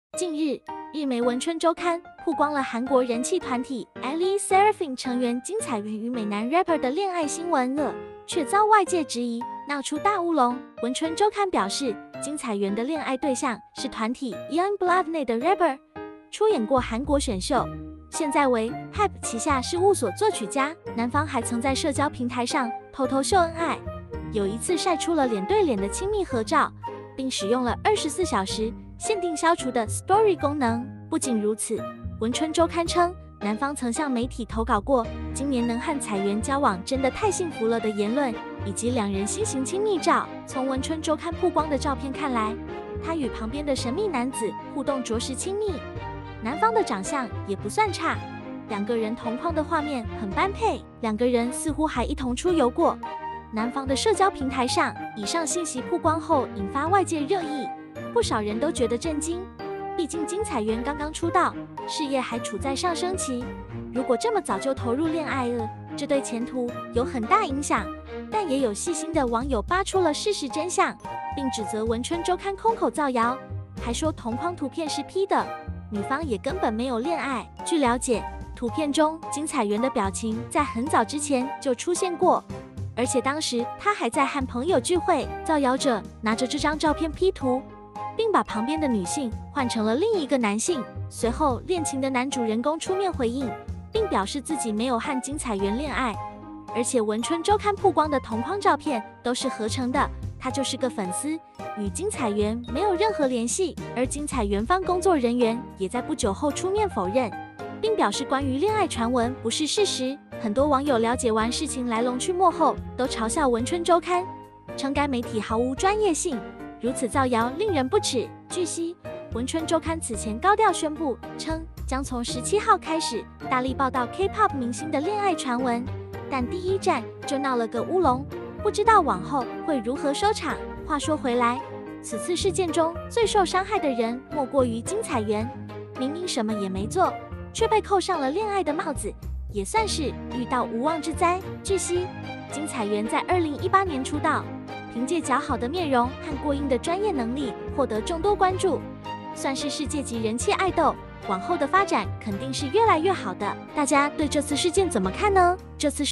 LE SSERAFIM金采源被日媒曝戀情！與美男嘟嘴貼臉，男方最新回應嚟了#娛樂礦工#LE SSERAFIM#金采源#k-pop
近日，日媒文春周刊曝光了韓國人氣愛豆金采源與美男rapper嘅戀愛新聞，卻遭外界質疑鬧出大烏龍。
文案：方方土 后期：丢丢